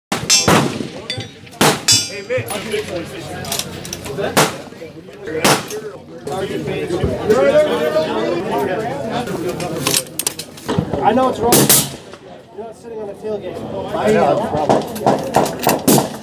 1000 Yard Shot | Aero Precision Solus Competition Rifle
This Aero Precision Solus Competition Bolt-Action Rifle is chambered in 7mm prc.